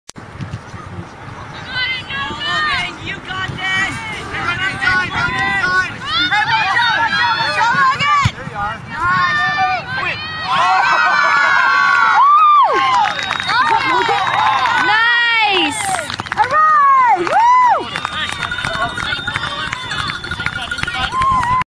unbelievable goal!
Logan Hardy makes the impossible goal!